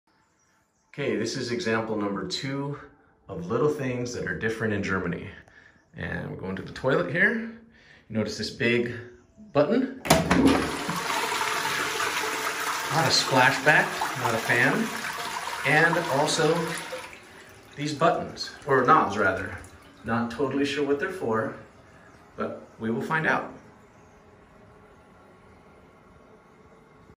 Little Things In Germany 🇩🇪 That Are Different S1 E2 “Toilets 🚽 “
No the large button panel and the splash of water